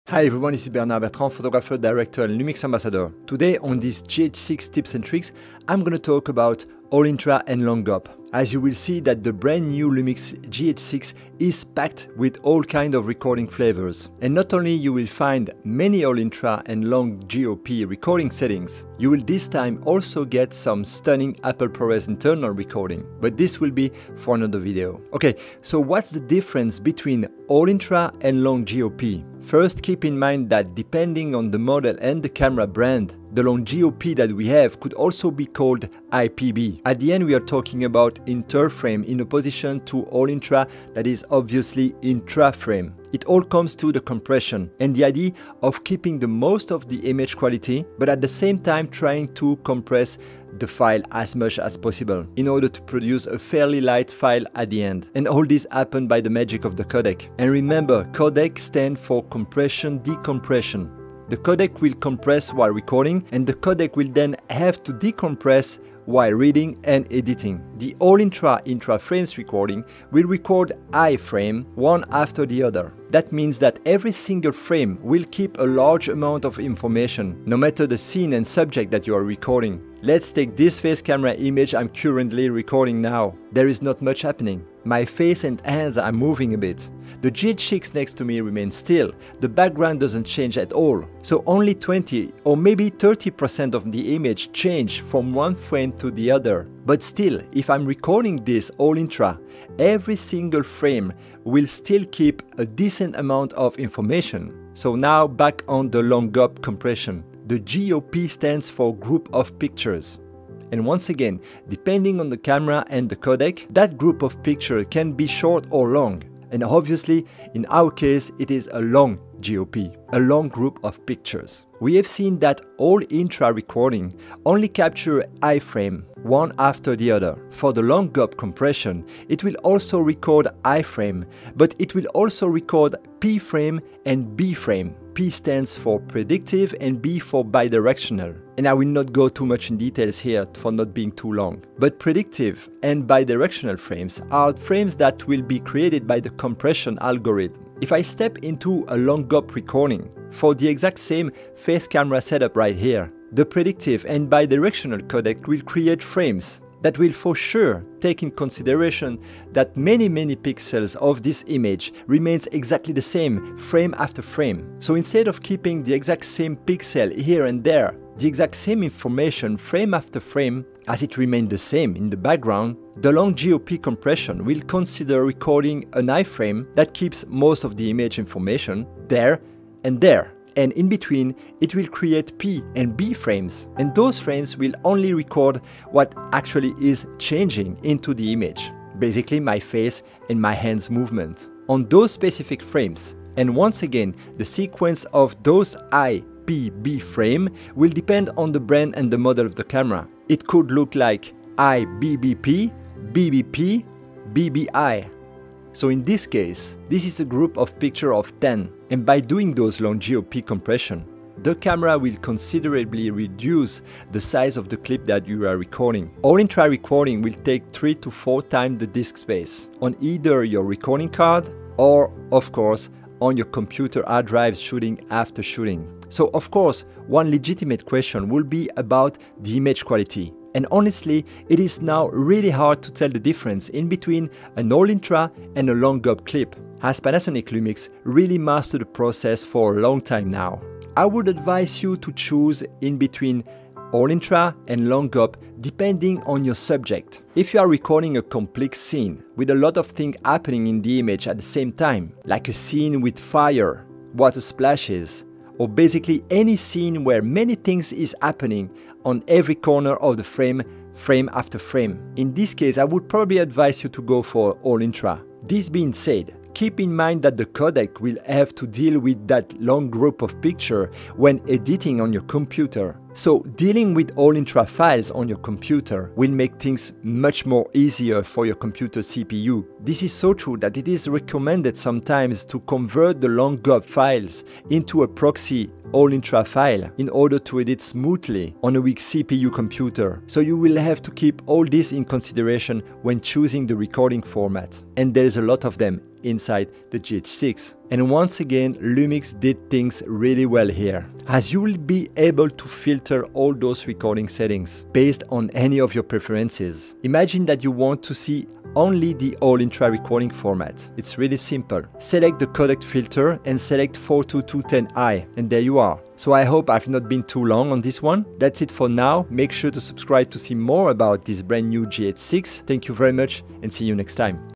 All Intra vs. Long GOP | LUMIX Academy | GH6
In this LUMIX Academy video, LUMIX Ambassador Bernard Bertrand is talking about the different video codecs: All Intra and Long GOP.
When to use which codec and what are the main differences ?